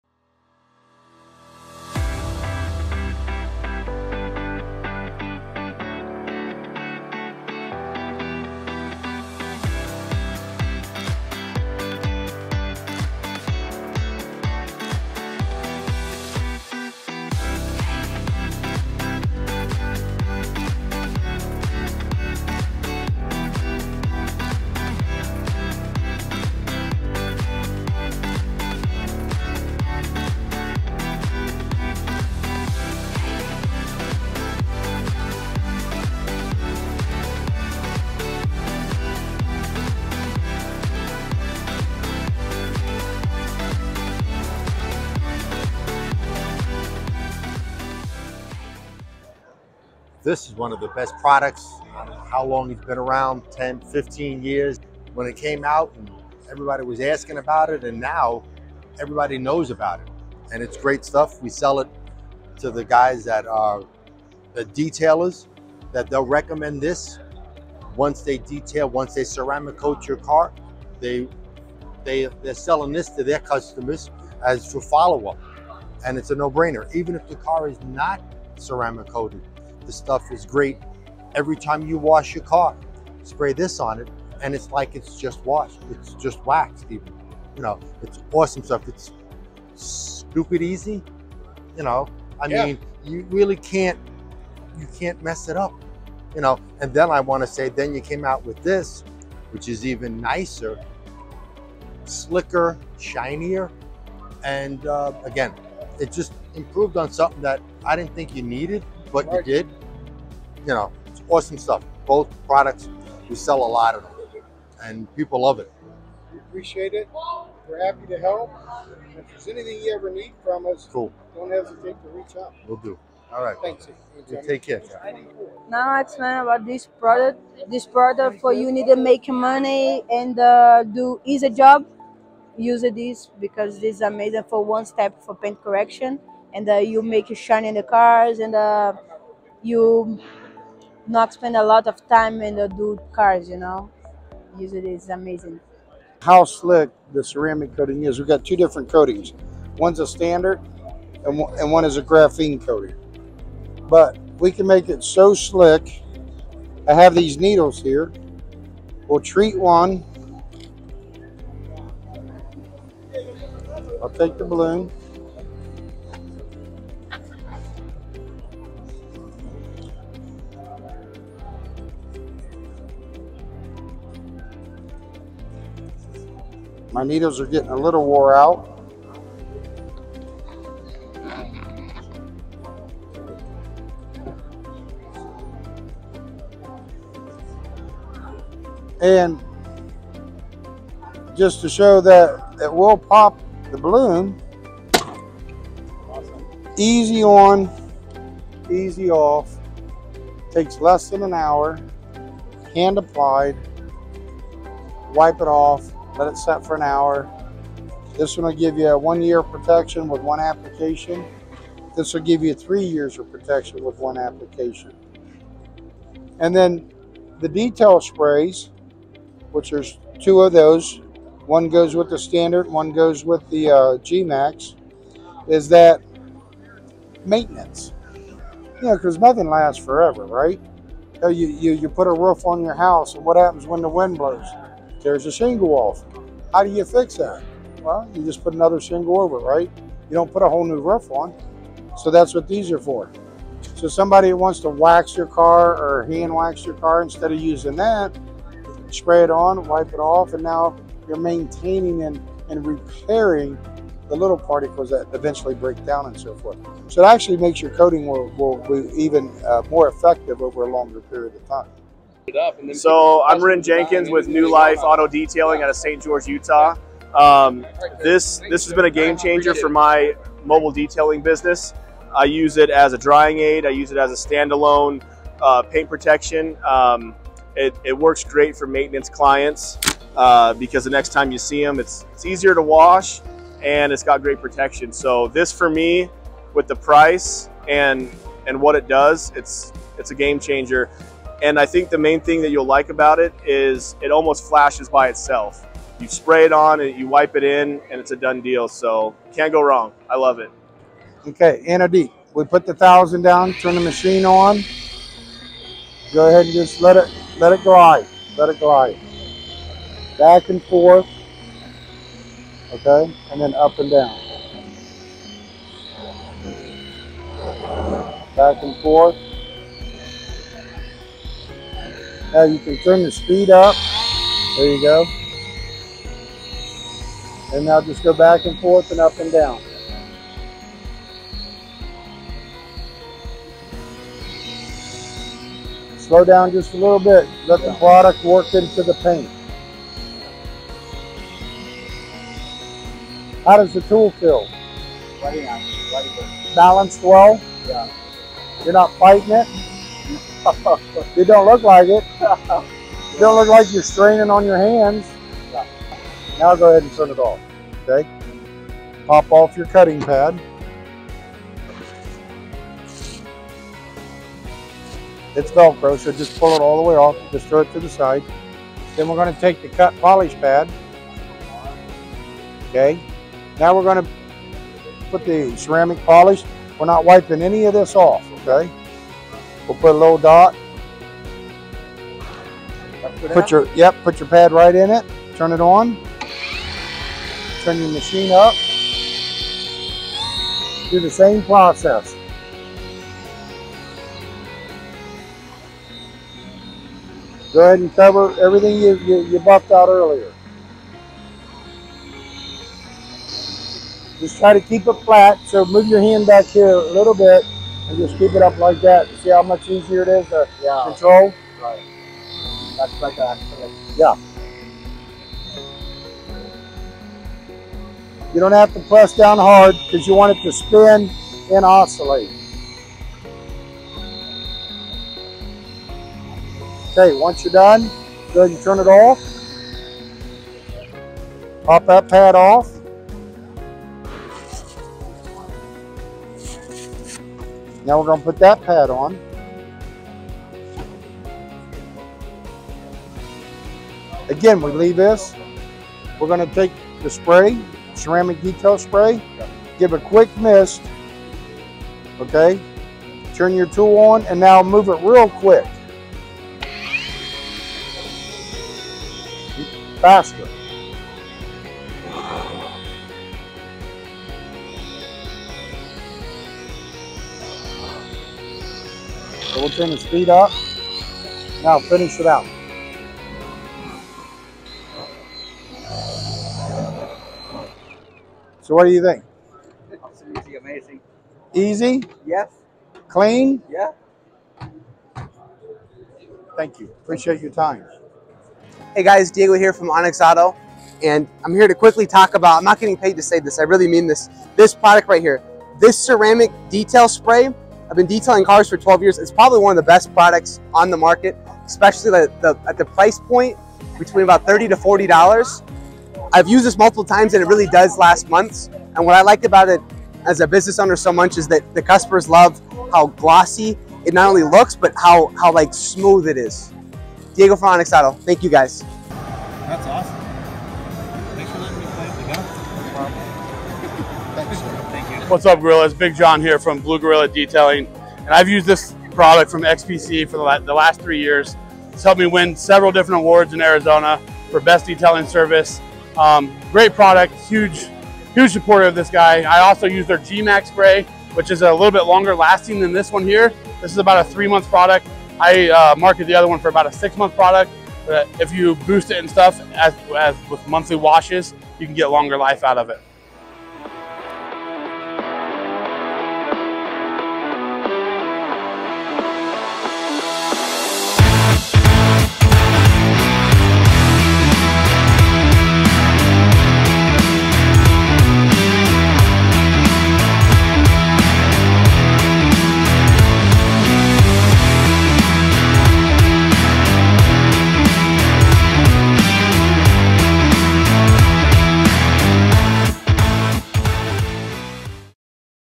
SEMA 2022
Had a great time demoing some of our products at SEMA 2022! Listen to what some of our customers had to say about XPC3 Ceramic and G-MAX! Just to name a few!